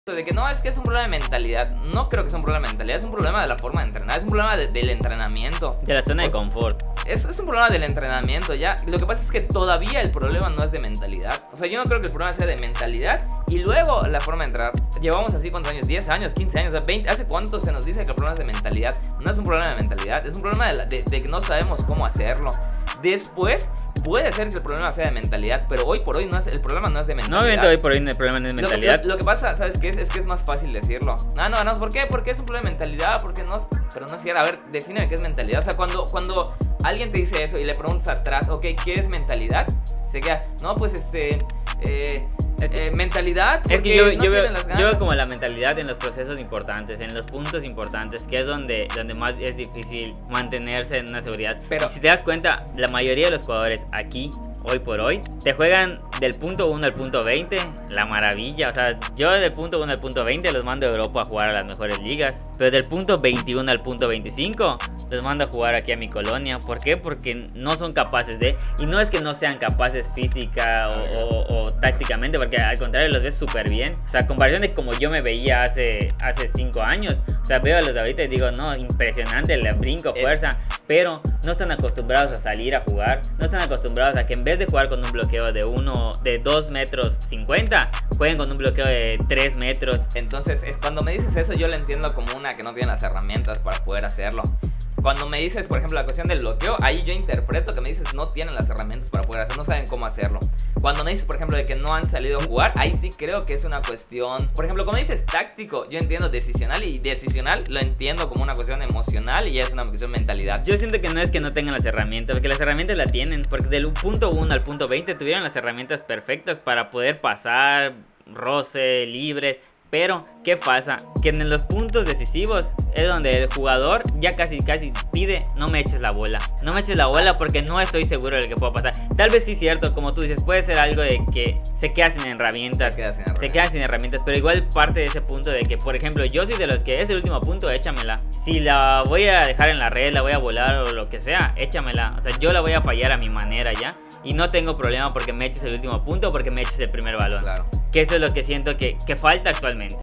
¿Realmente es un problema de mentalidad?
Muchas veces decimos que es un problema de mentalidad porque es mucho más fácil decir eso, que profundizar en las circunstancias que están detrás y alrededor de una derrota.